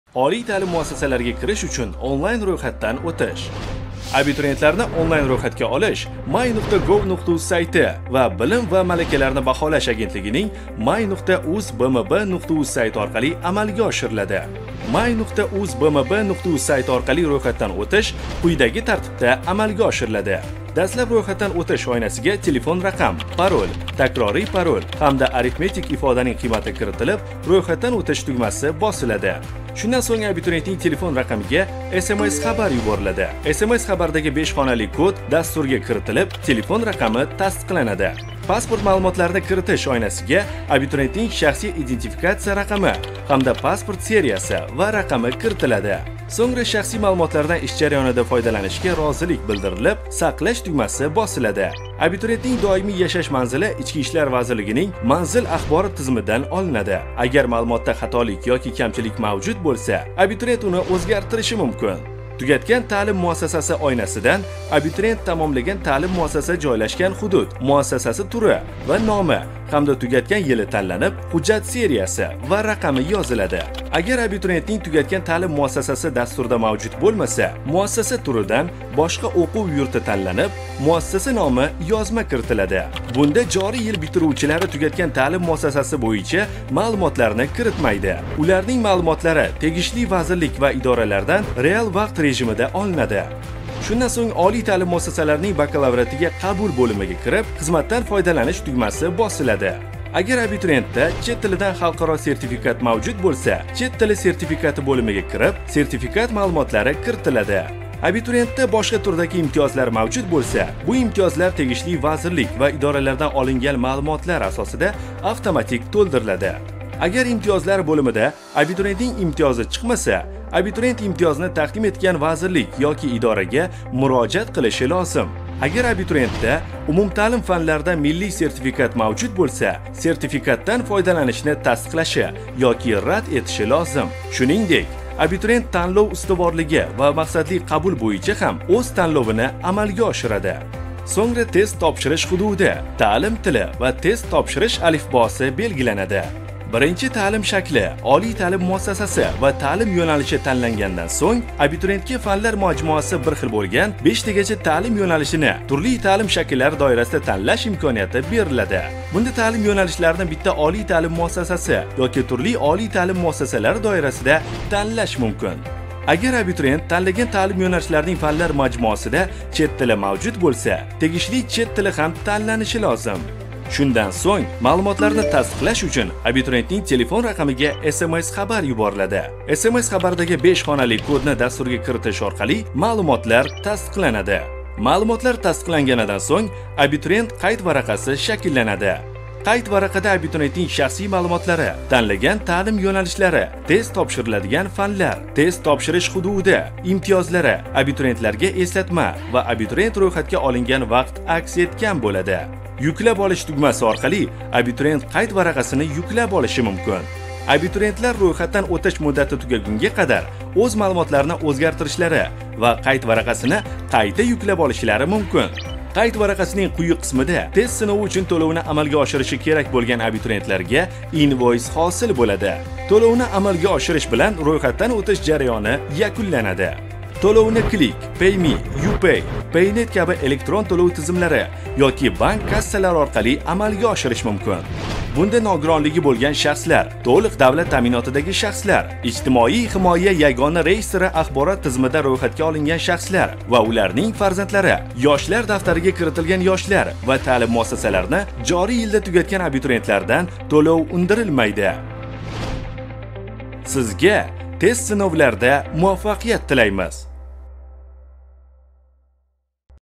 1 Ro‘yxatdan o‘tish surdotarjimon bilan~1
Oliy ta’lim muassasalariga kirish uchun onlayn ro‘yxatdan o‘tish bo‘yicha videoqo‘llanma